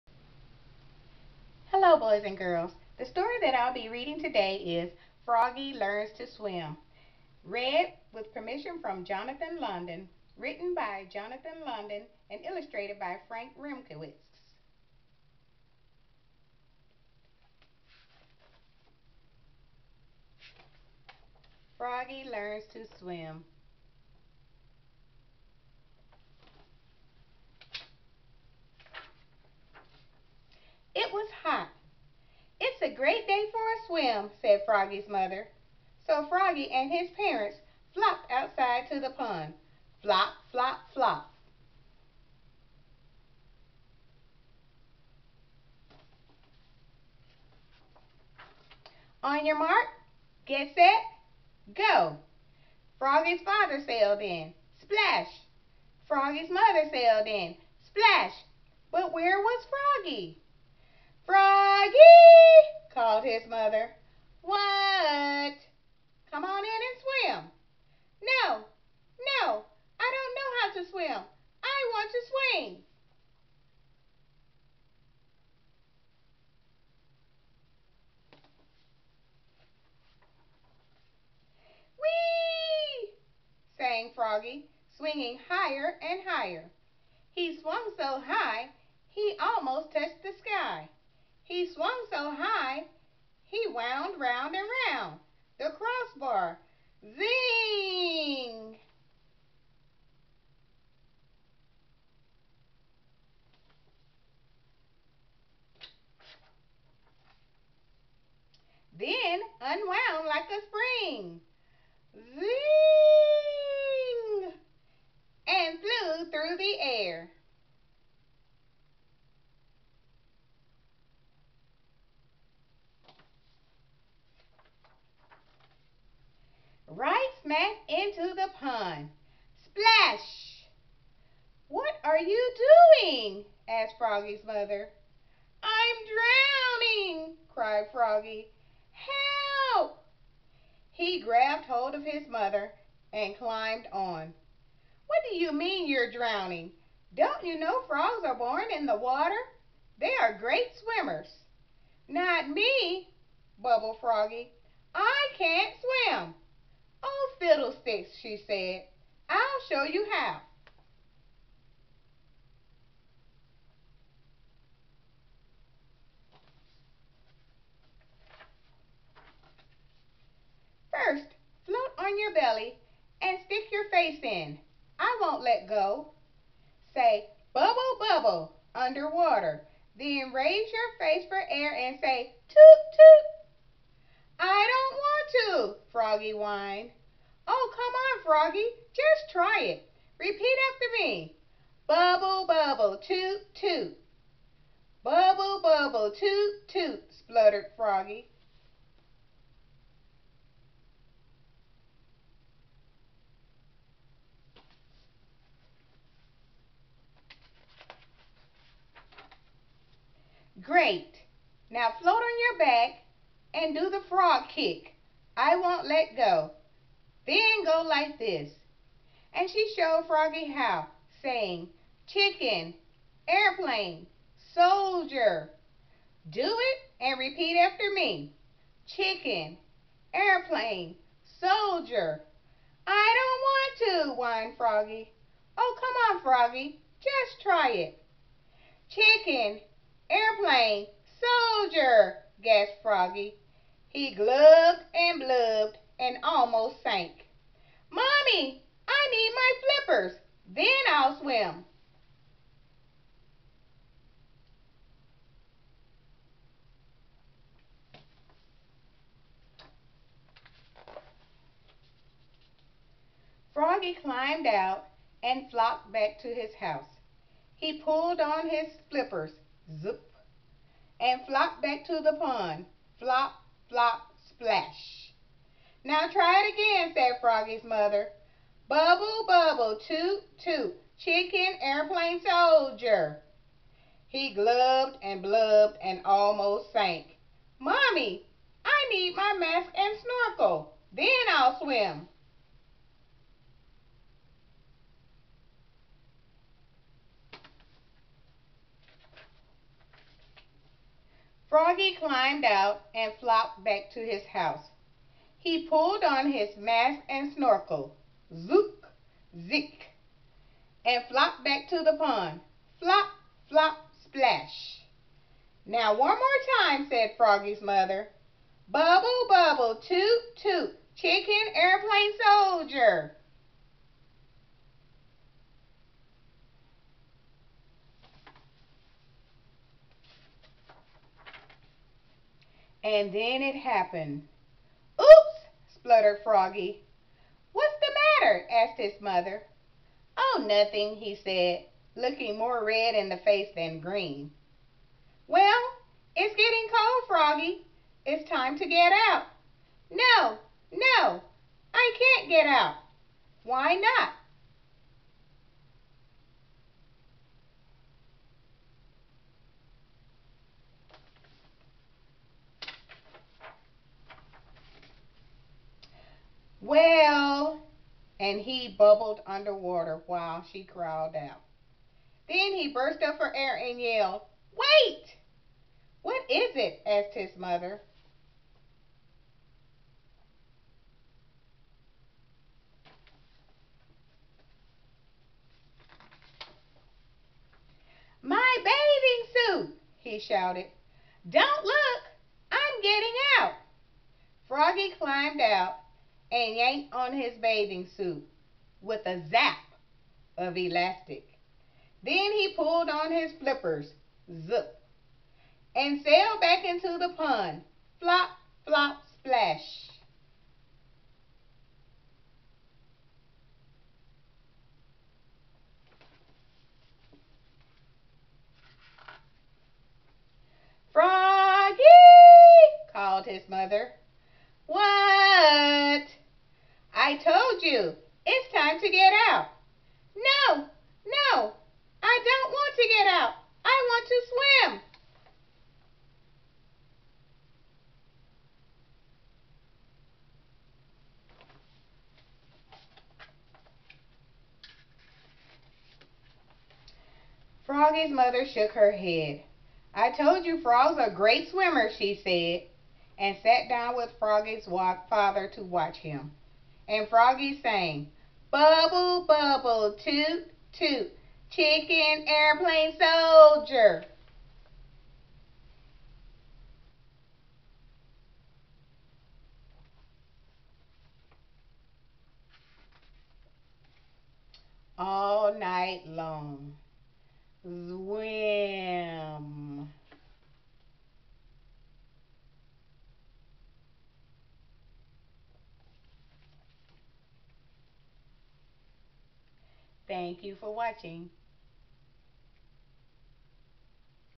FROGGY LEARNS TO SWIM
Frogs are supposed to be great swimmers, but not Froggy.  He is afraid of the water.    However, with practice, encouragement, and a silly song, he becomes an expert swimmer.